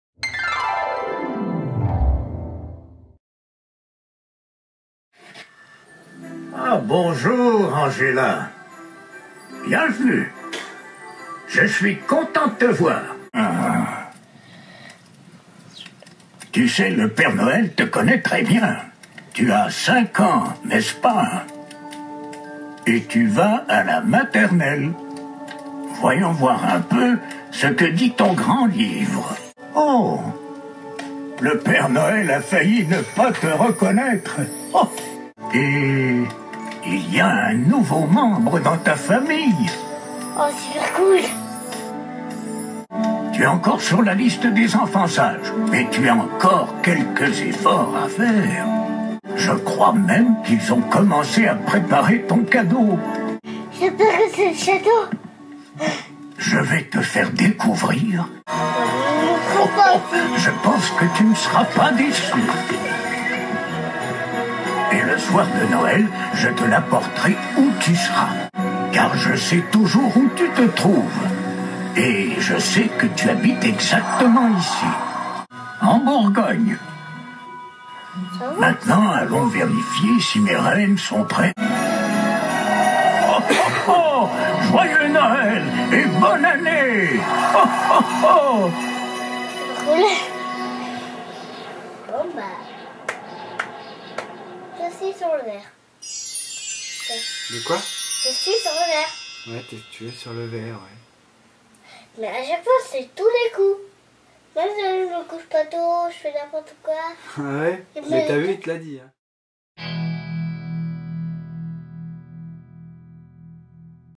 Message papa Noël 2012
Petit message du papa Noël pour Angela ! Réduit généreusement bien sur!!
Les réaction d'enfants c est quand même le plus beaux des cadeaux des parents